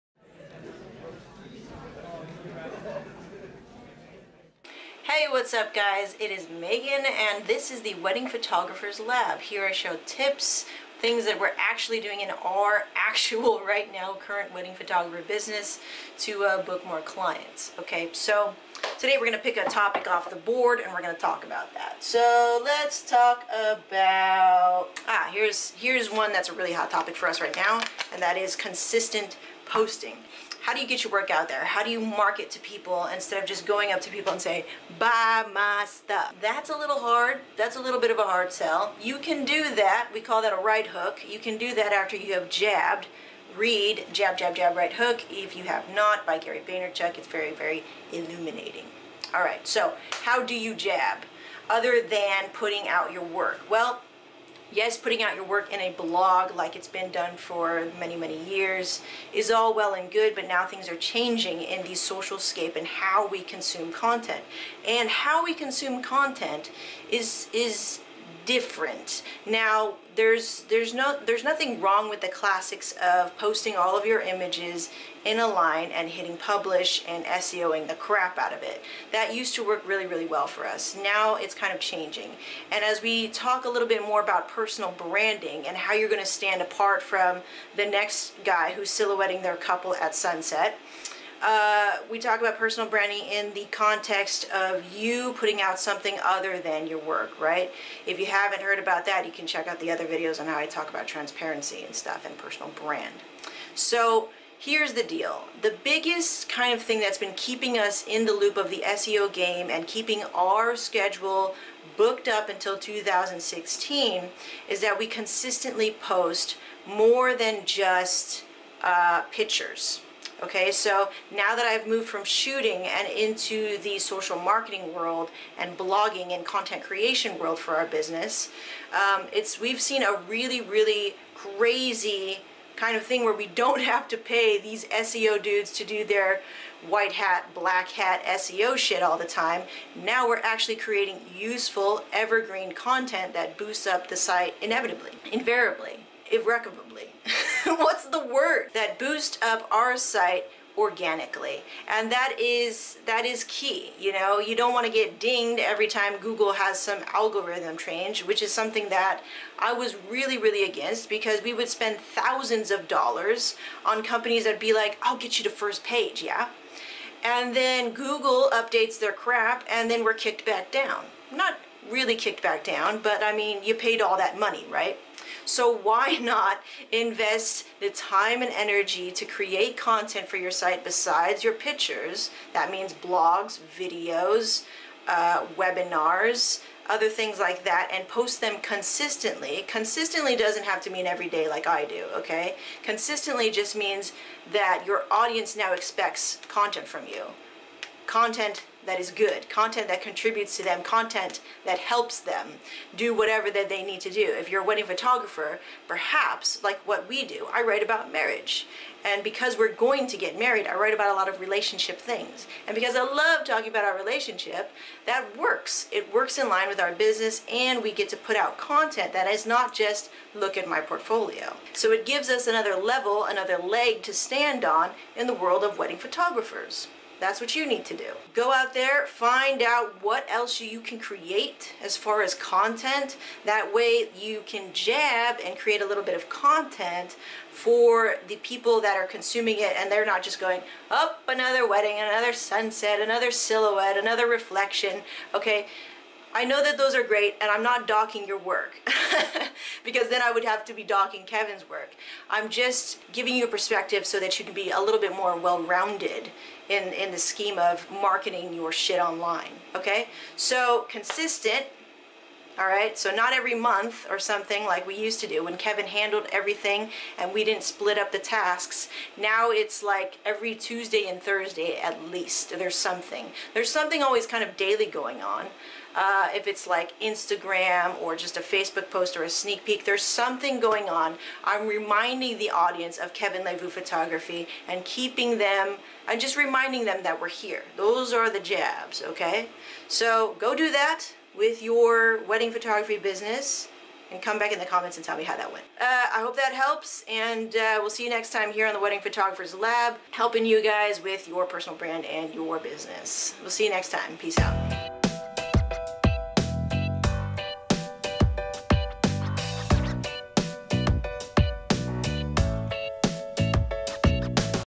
How Do You Market Your Work Besides a Portfolio?
Learn how to market your work besides posting your portfolio or pictures. Utilize your personal brand to stand out from the pack and create content that your audience wants. Personal branding in this age is a subtle thing. Or at least it can be. Learn how to jab in online social. SEO and photos still is the base of our blogging scheme, however, in order to stand out to our clients, we have all kinds of other content that is transparent to ourselves as people and photographers.